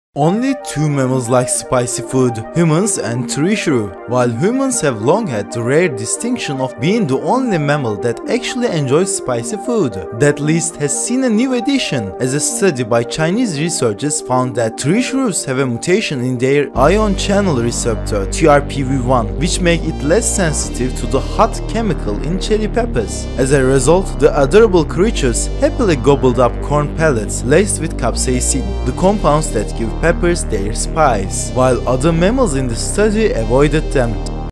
Spicy Food: A Taste Shared by Humans and Tree Shrews
Delve into the intriguing world of spicy cuisine and discover the surprising connection between humans and tree shrews. Uncover the evolutionary reasons behind our love for spicy flavors and how it sets us apart from other mammals.

Producer :  İsmet Canpolat